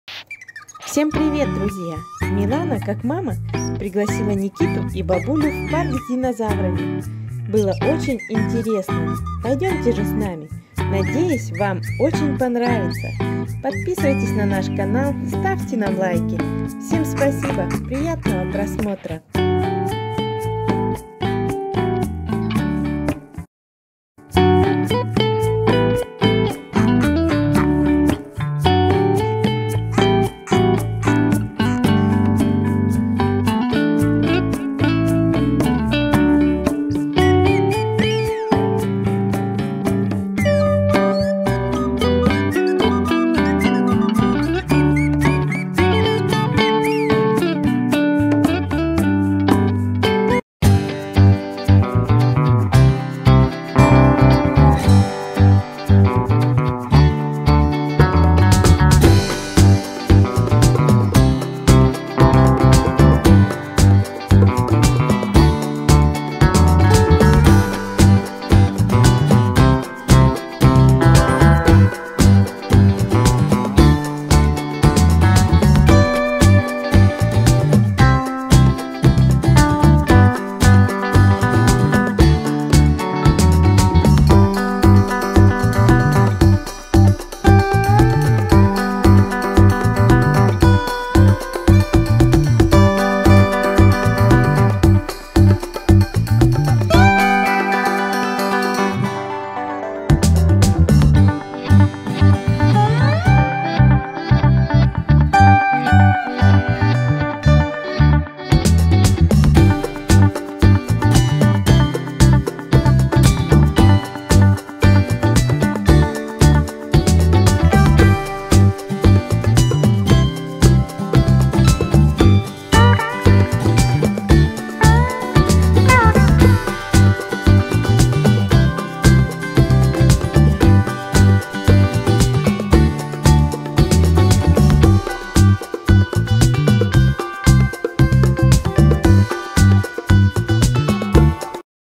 Дети подружились с Динозавром! Милана КАК МАМА пригласила Никиту в парк Интересное видео для детей
Дети подружились с Динозавром! Наш выходной день Милана КАК МАМА пригласила Никитоса в парк динозавров в Ярославле Влог . Детское видео о том как дети подружились с динозавром. Милана КАК МАМА долго думала куда можно пойти с детьми и  пригласила Никиту в парк динозавров в Ярославле. Детям очень понравилось в парке.

Children made friends with the Dinosaur! Our day off in Milan AS MOTHER invited Nikitos to the dinosaur park in Yaroslavl Vlog. Children's video about how children made friends with a dinosaur. Milana AS MOTHER invited Nikita to the dinosaur park in Yaroslavl. Children really enjoyed the park.

Boo boo story from Milana Ребёнок как мама лечит бэби борн!